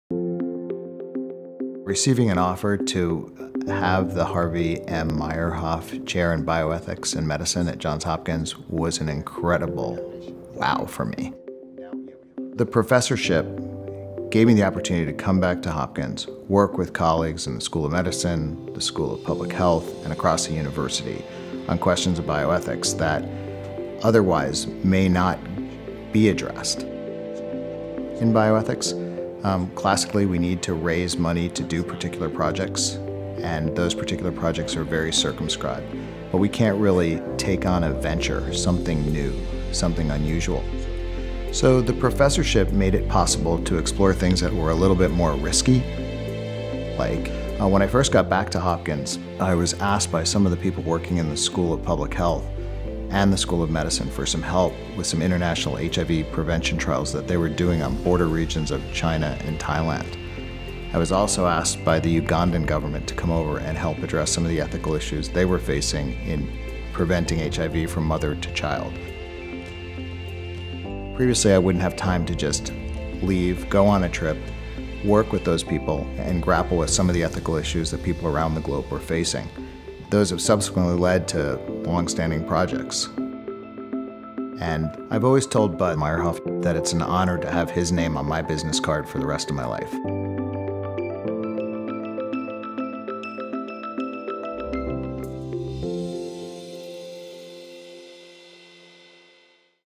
Grappling with the bioethics of HIV around the world
Jeremy Sugarman – deputy director for medicine of the Johns Hopkins Berman Institute of Bioethics and the Harvey "Bud" M. Meyerhoff Professor of Bioethics and Medicine – takes on ethical challenges from HIV prevention to organ transplants. to learn more.